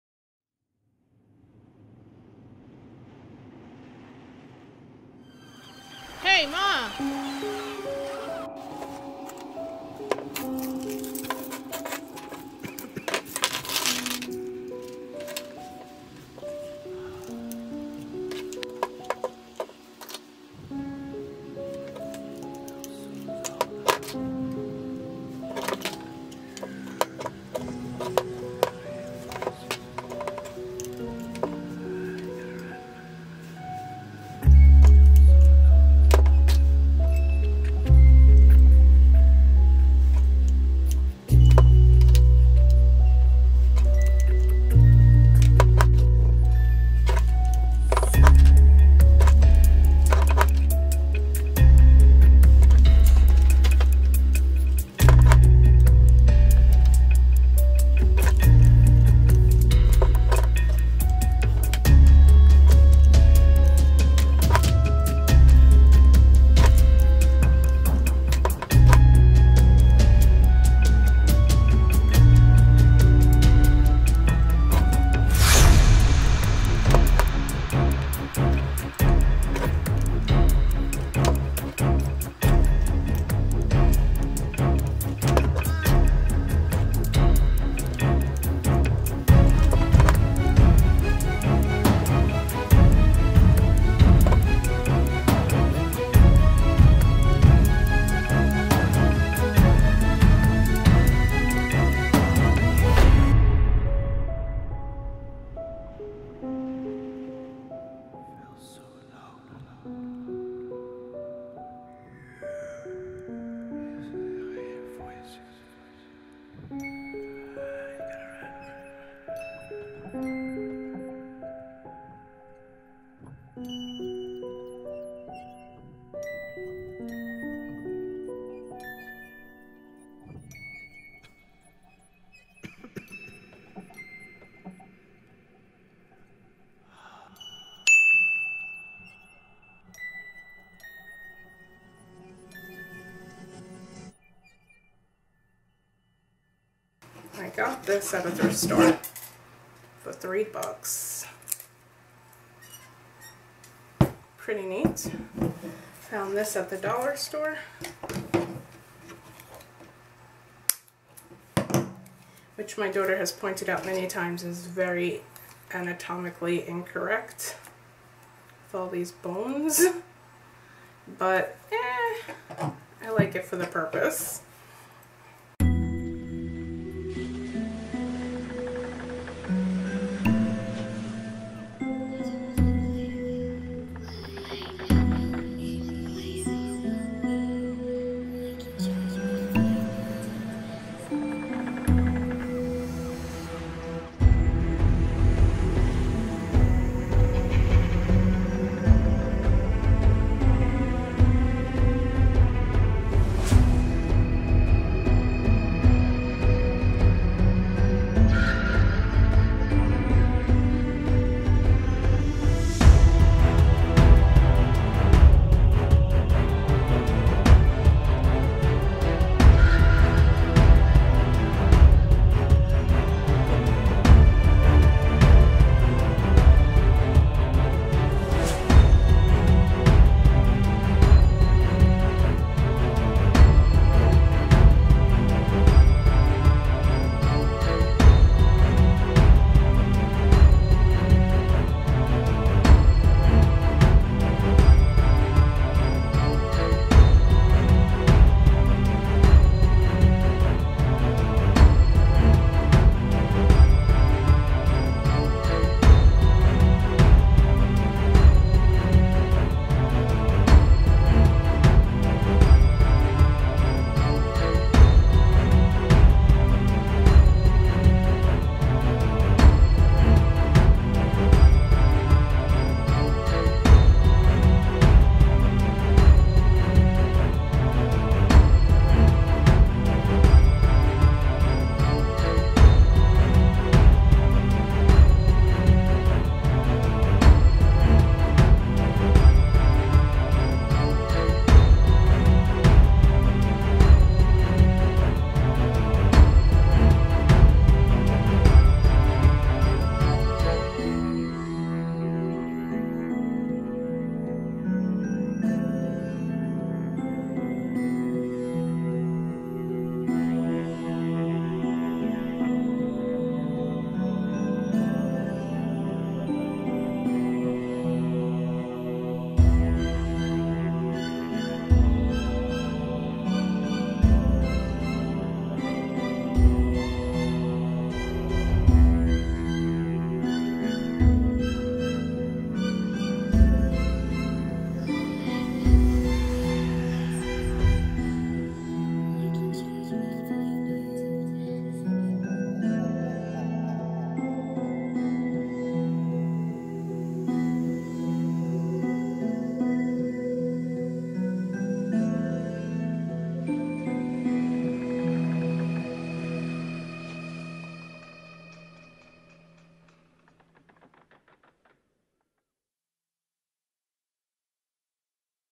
Easy Fireplace Mantel 🎃 Halloween Decorations
Decorate your Fireplace Mantel for Halloween with these fast and easy crafts. I was able to create this Fall decor for under $10

The story behind this is I was visiting my daughter. She just started grad school in a new state, and is currently renting a house with a fireplace. But she had no idea how to decorate her mantel for Halloween, so she offered it up to me for a crafting video! Sharp-eyed viewers might recognize the photos above the mantel as photos of my daughter’s dogs!

And of course our website...